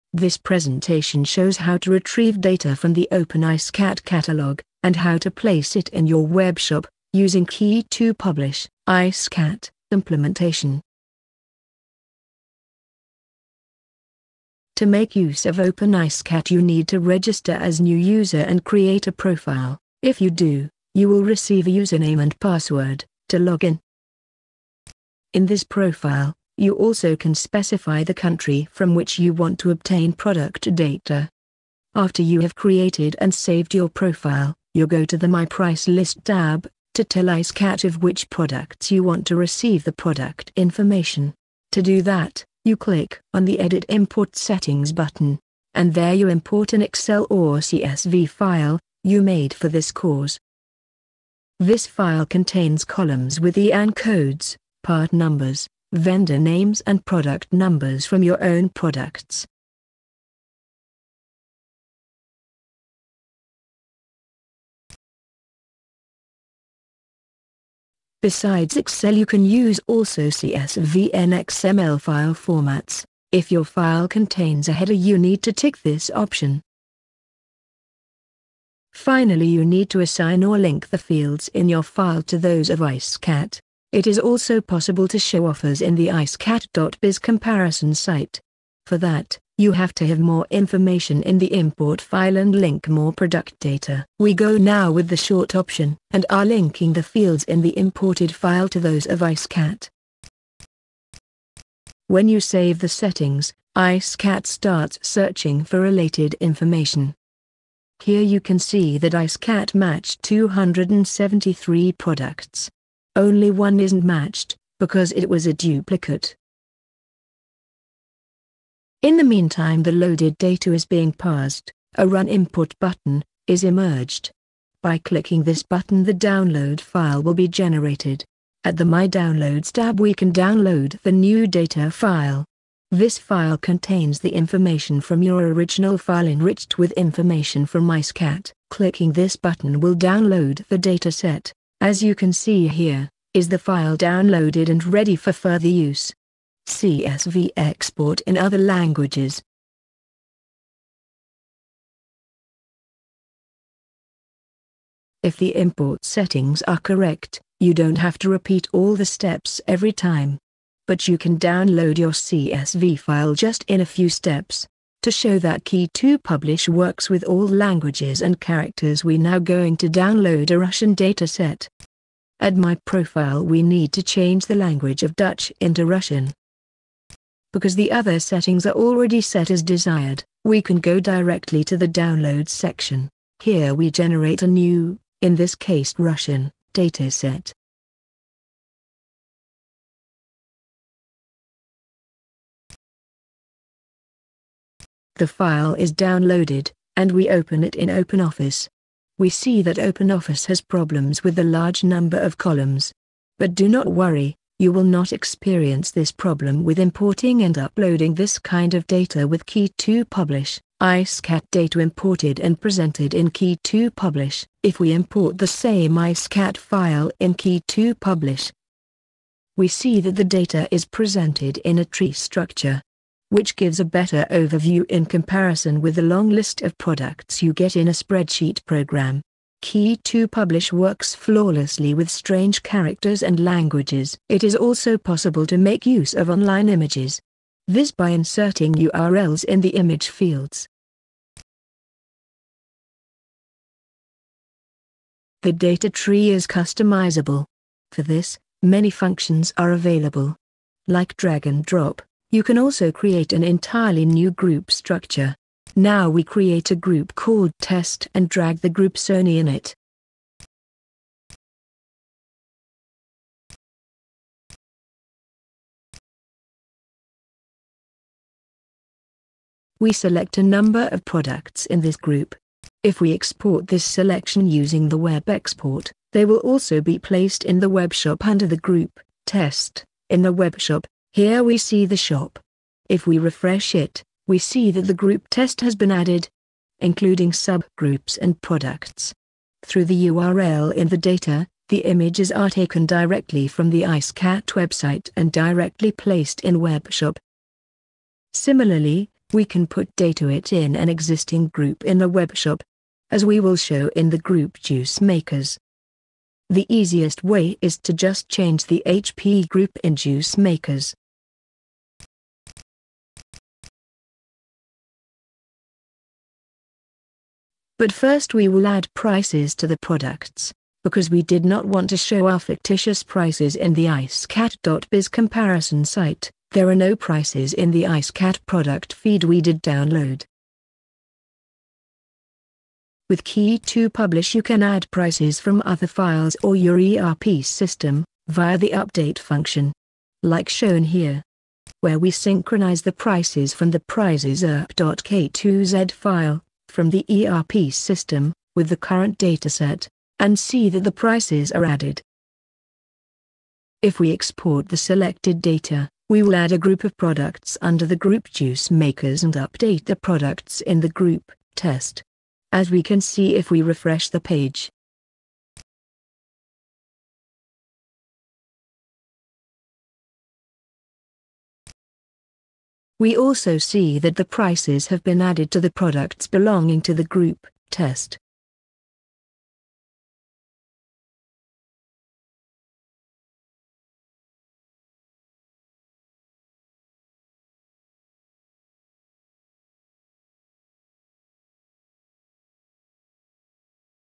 Upload Icecat data to a webshop using Ke2Publish
This presentation shows how to retrieve data from the open icecat catalog, and how to place it in your webshop, using Key2Publish